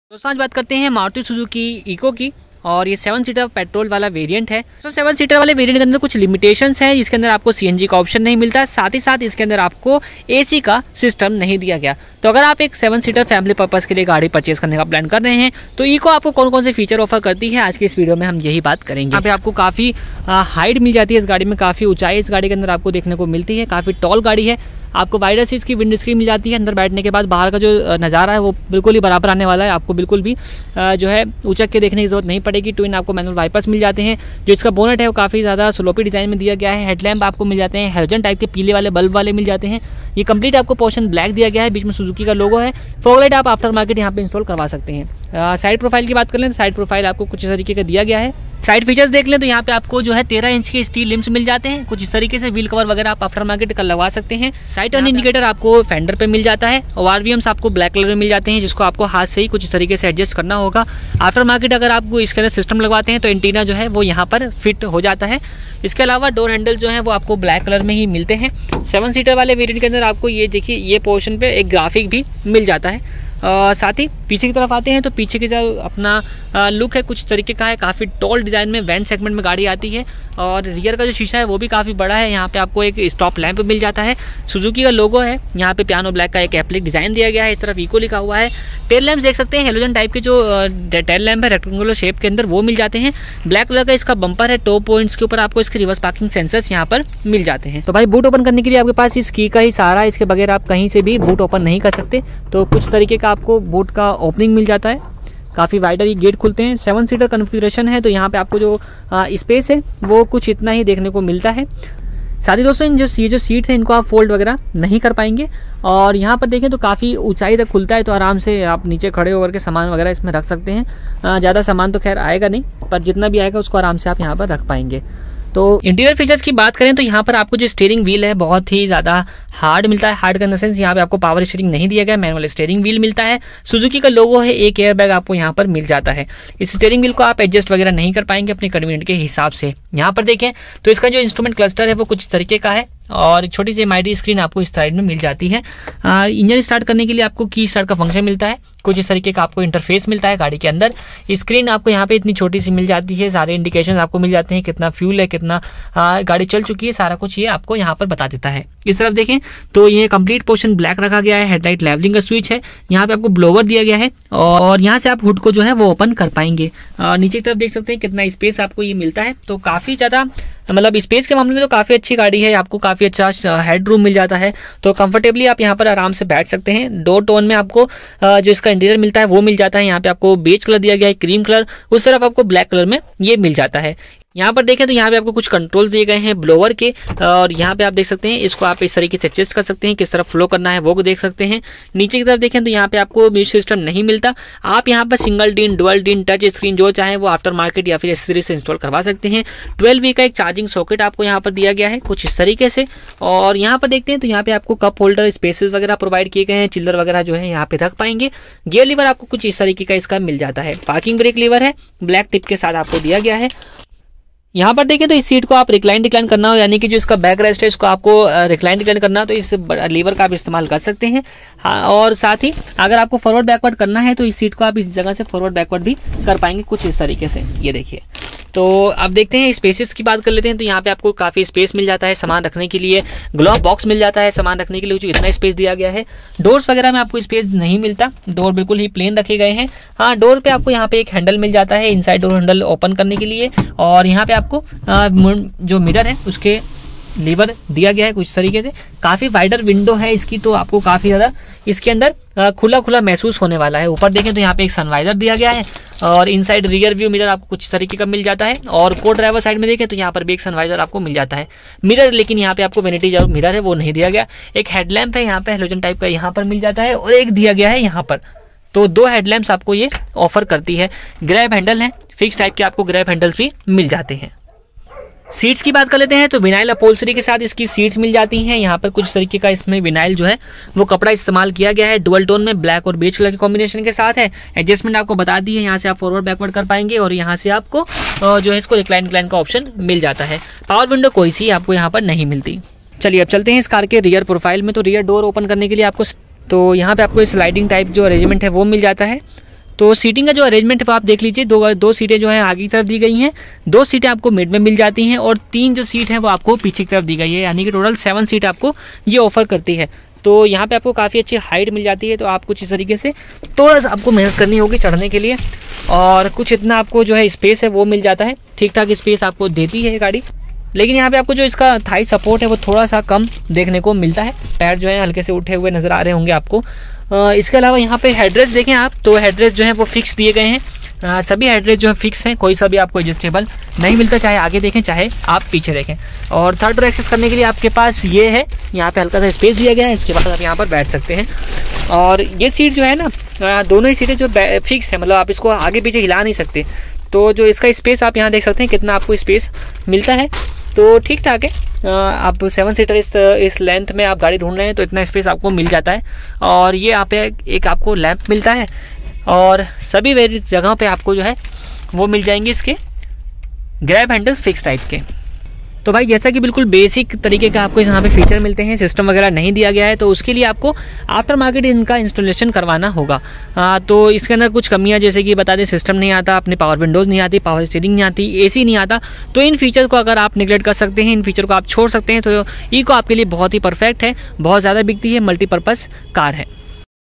2021 Eeco 7 Seater || Detailed Walk-Around Review, Interior, Exterior & Features
2021 Eeco 7 Seater || Detailed Walk-Around Review, Interior, Exterior & Features